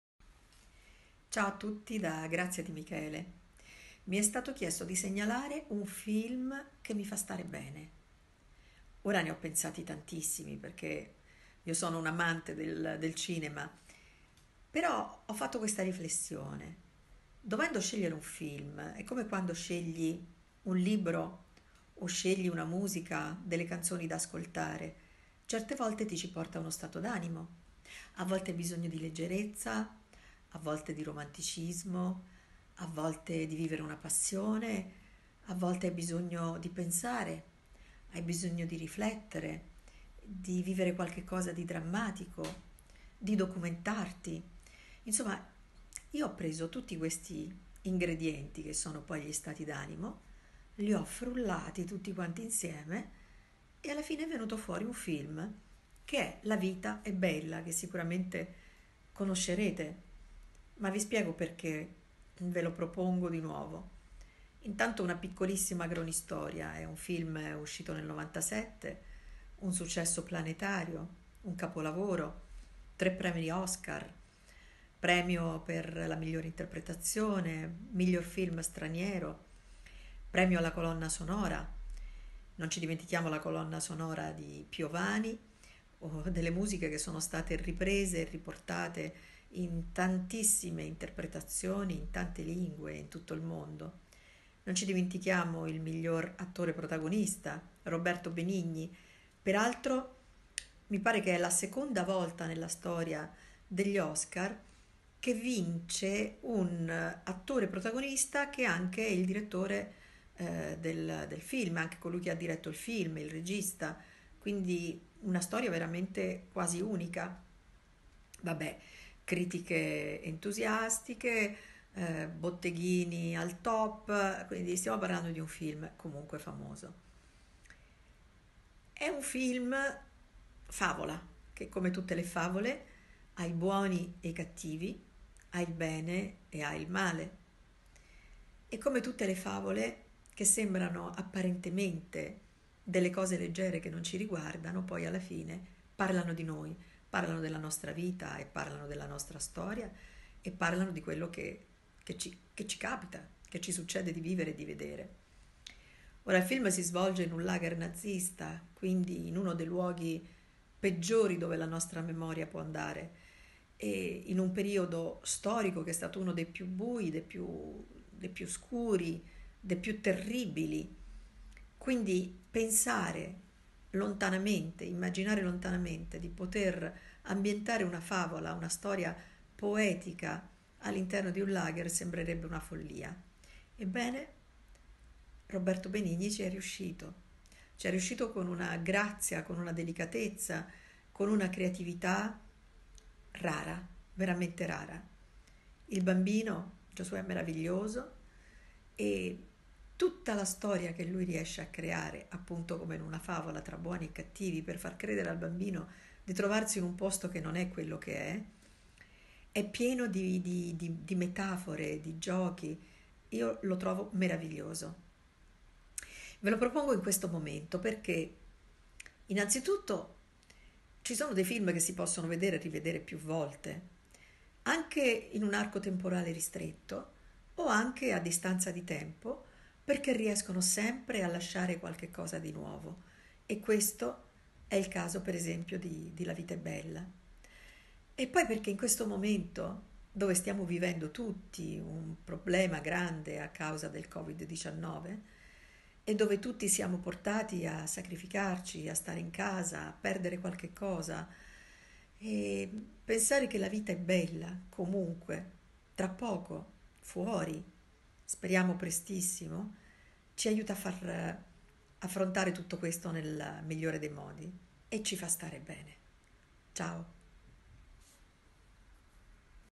Il film che mi fa stare bene: Grazia Di Michele, cantautrice, docente di musicoterapia.
Scopri quale film hanno scelto tutti gli amici di Medicinema Italia. Vai alla pagina